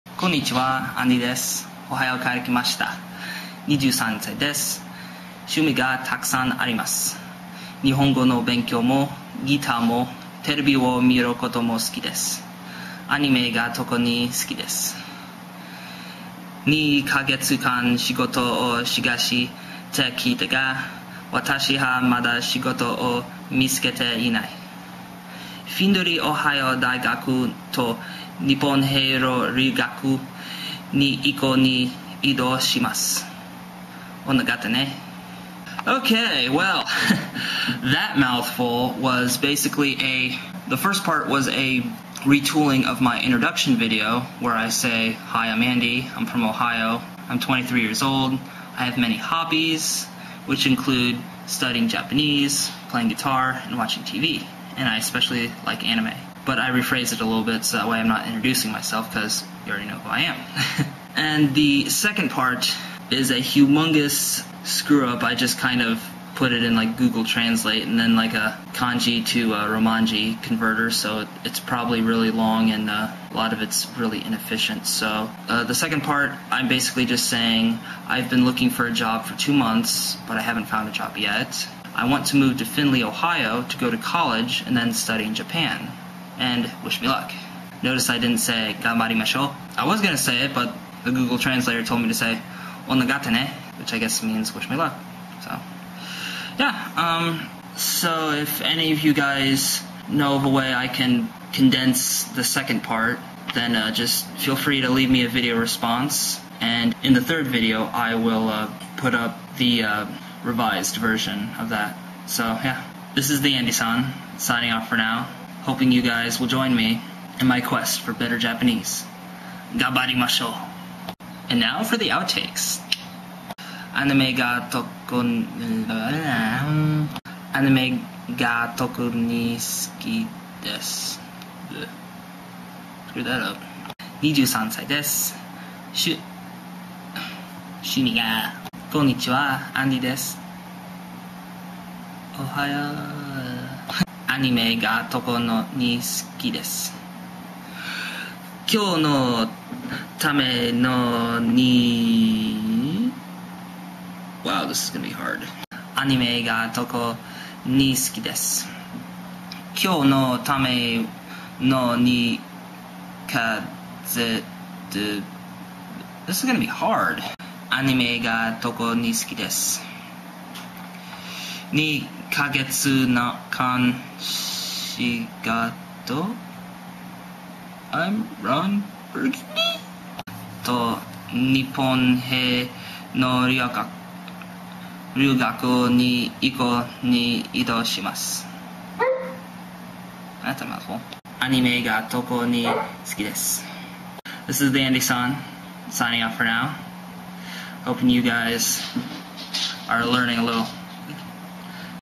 College Goals - J-Vid 02 - TheAndySan
In this video, I first correct parts of my introduction video and then I talk about how I plan to come over to Japan via a college study-abroad program. Here's the dialog in Romanji and English:

Konnichiwa, Andy desu.
(Hi, I'm Andy)

Ohio kara kimashita.
(I'm fron Ohio)

Nijuusan-sai desu.

Shumi ga takusan arimasu.
(I have many hobbies)

Nihongo no benkyou mo, gita mo, terebi wo miru koto mo suki desu.
(I like Japanese studies, guitar, and watching TV)

Anime ga toku ni suki desu.
(I especially like anime)

Nii kagetsu kan shigoto o sagashi te ki ta ga, watashi ha mada shigoto o mitsuke te i nai.
(I've been looking for a job for two months, but I haven't found a job yet.)

Findoree ohaio daigaku to nippon he no ryuugaku ni iku ni idou shi masu.
(I want to move to Findlay Ohio to go to college and study in Japan.)

O negatte ne!

TheAndySan

P.S: The next Japanese video that I do will have the Kanji accompanying the dialog so that people who can read it can get a better understanding of what I'm trying to say (which includes my native Japanese audience as well as foreigners in Japan).